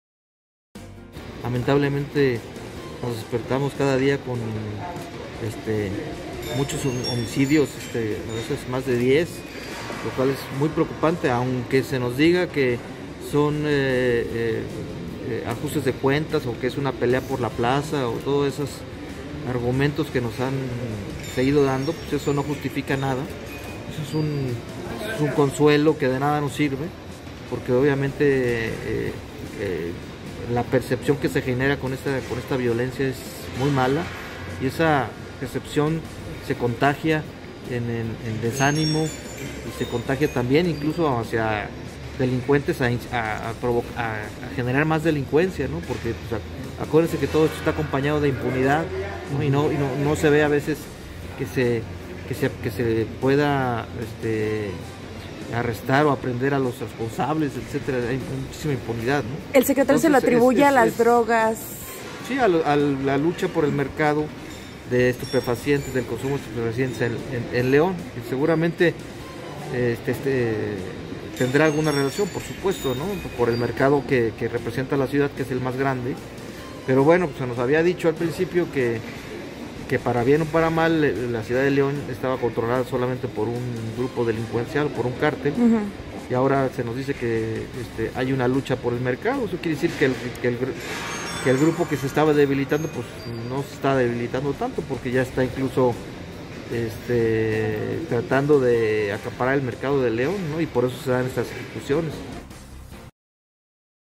Nada justifica repunte de violencia en León: CCEL
El empresario señaló la necesidad de que la autoridad explique el repunte en homicidios en febrero